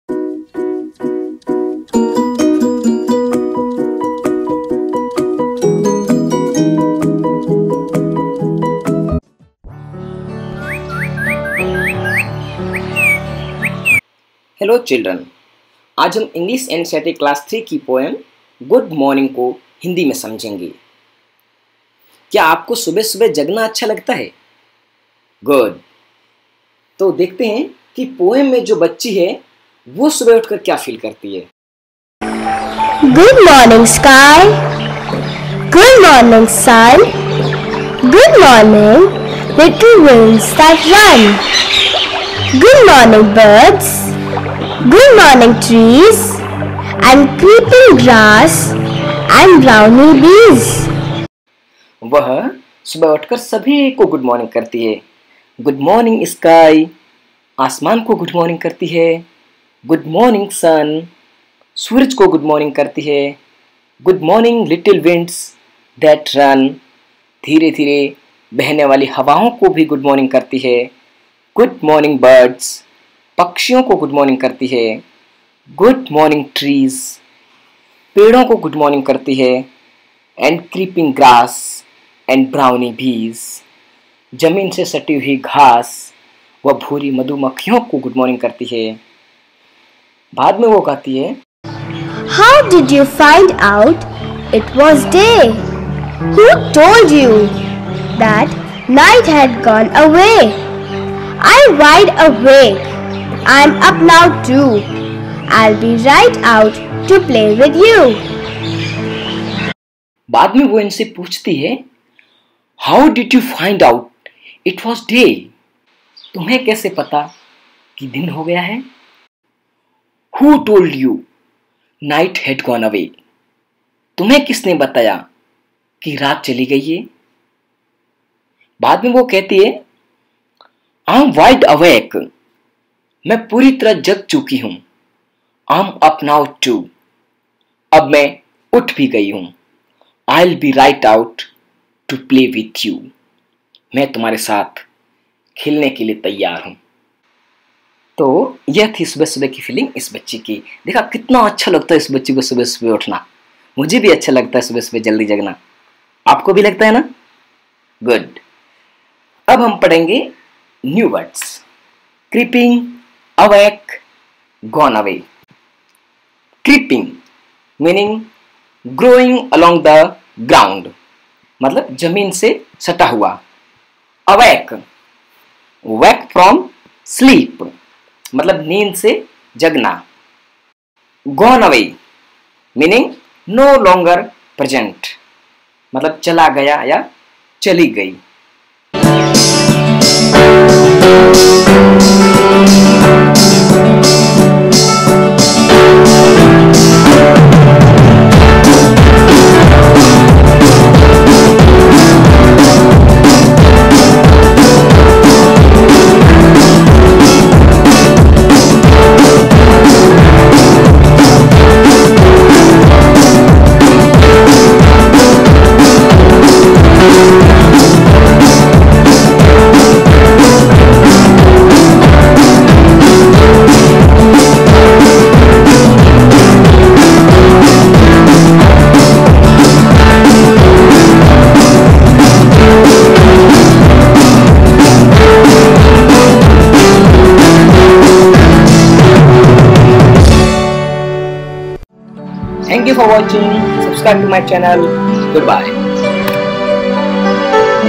1(3). Good morning ( Class 3 English ) / Part-3 / Explanation in hindi
Explanation in Hindi with music.

Good Morning, sky;
Good Morning, sun;
Good Morning, little winds that run! Good Morning, birds;
Good Morning, trees;
And creeping grass, and brownie bees! How did you find out it was day?
Who told you night had gone away? I’m wide awake;
I’m up now, too.
I’ll be right out to play with you!

Fannie R. Buchanan

new words
musical poem
poem with music